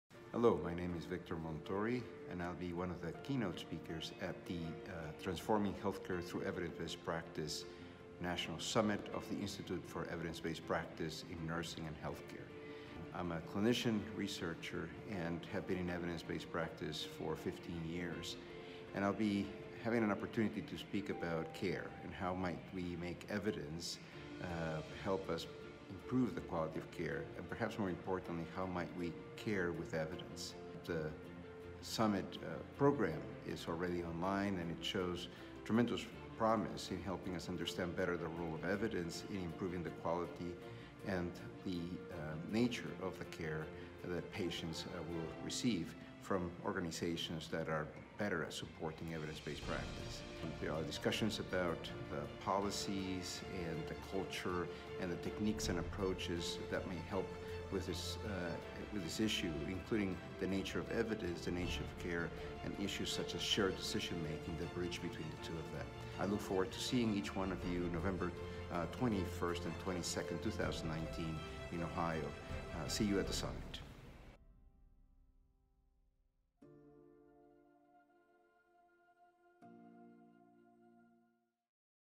Join Victor Montori at the 2019 EBP National Summit
Keynote speaker Victor Montori an endocrinologist and researcher from Mayo Clinic will discuss his expertise in evidence-based practice and shared decision making at the 2019 EBP National Summit hosted by the Helene Fuld National Institute for Evidence-based Practice in Nursing and Healthcare November 20-22 in Columbus. for more information and to register today, don't miss it!